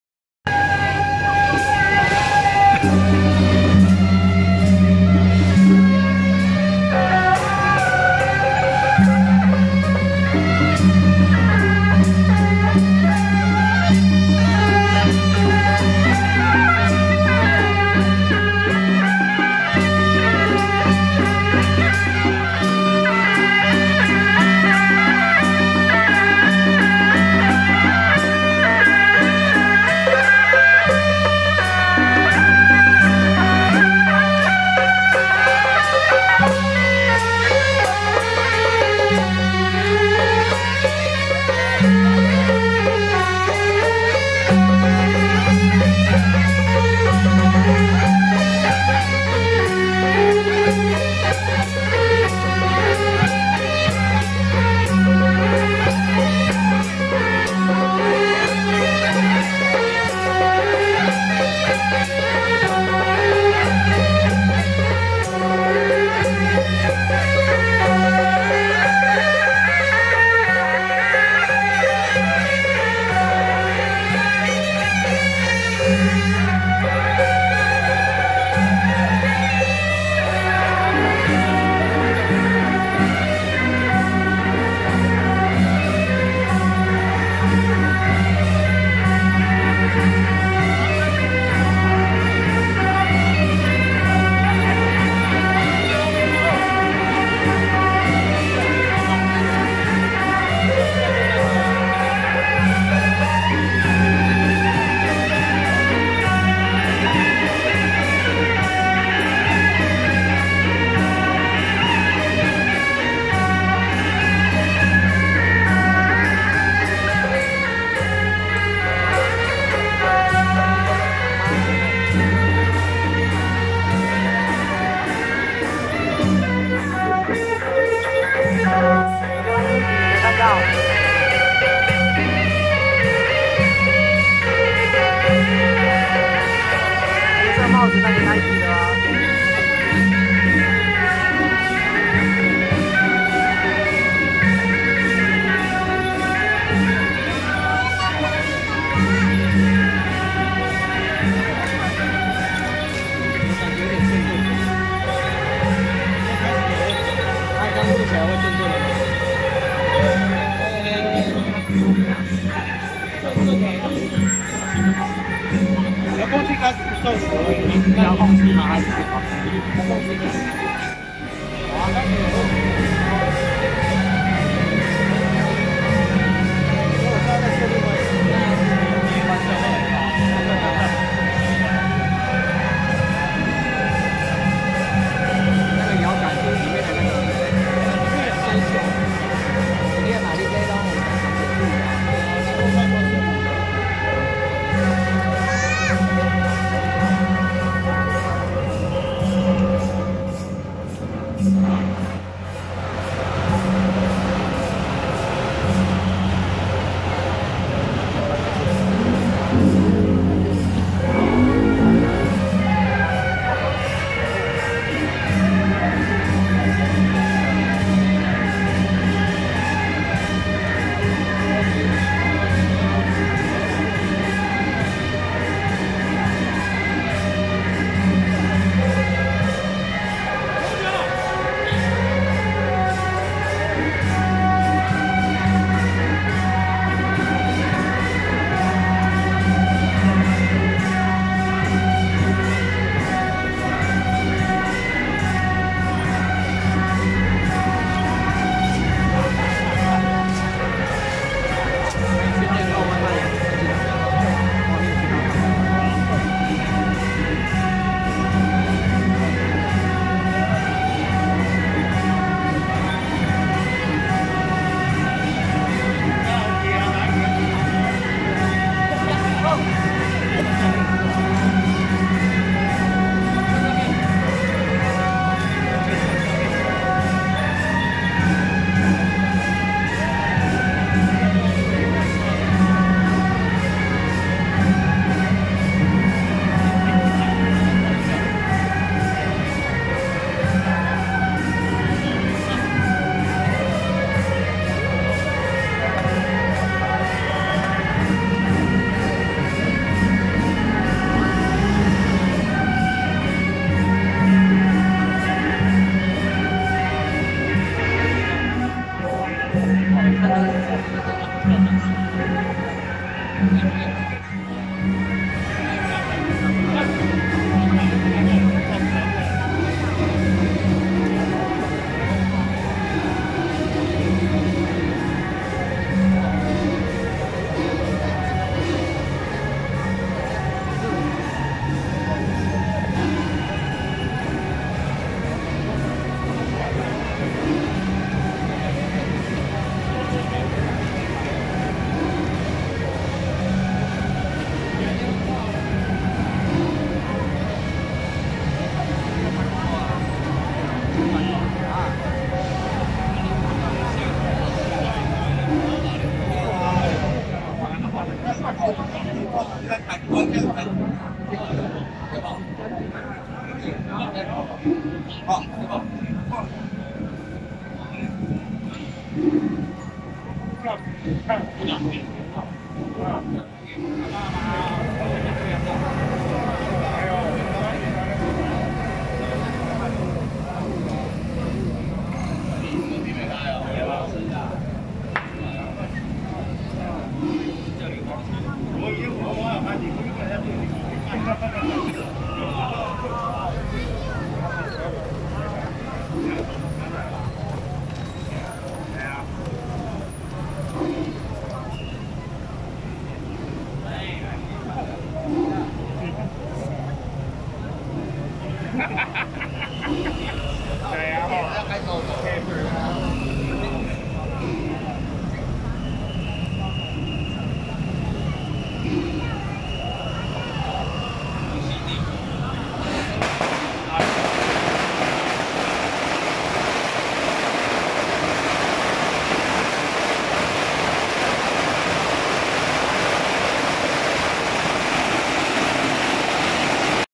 【台北龍興會舘+景美廣文軒+泰山池和堂-神將團】~108五股青天宮閻羅天子南巡謁祖進香回駕遶境
台北碧樂社-北管＆台北龍興會舘+景美廣文軒+泰山池和堂-神將團